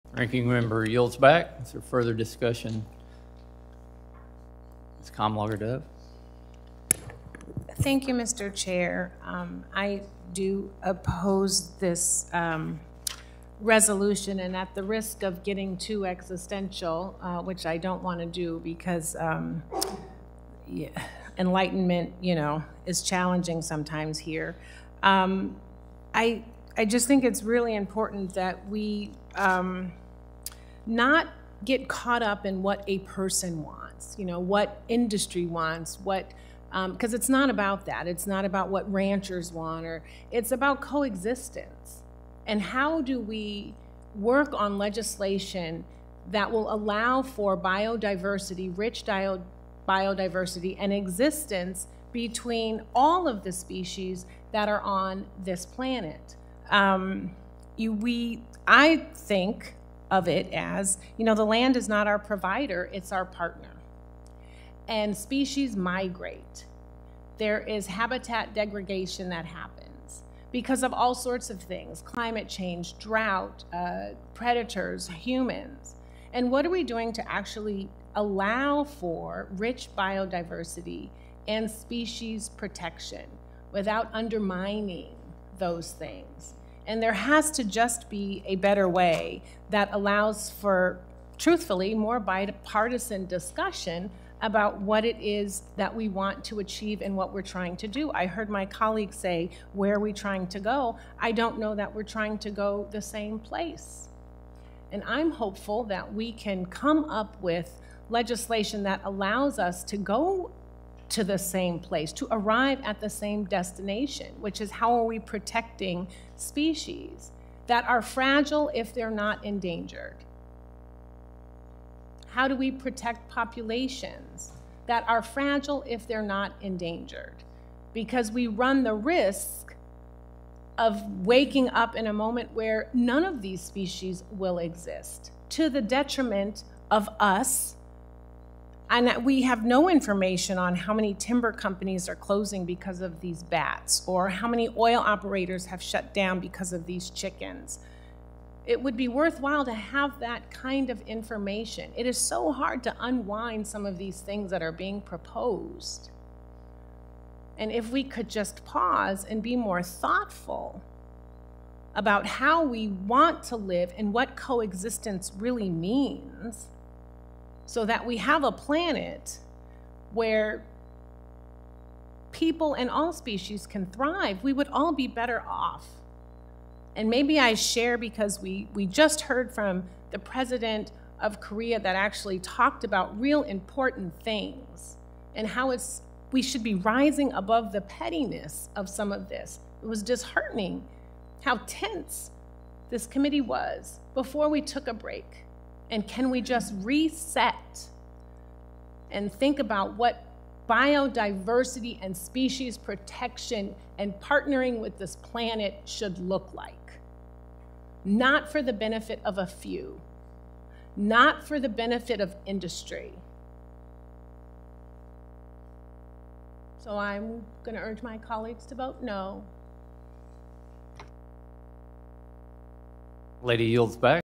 'If We Could Just Pause And Be More Thoughtful...': Kamlager-Dove Worries Over Species Protections
Rep. Sydney Kamlager-Dove (D-CA) spoke in opposition to a GOP-backed proposal to change the Endangered Species Act during a House Natural Resources Committee hearing.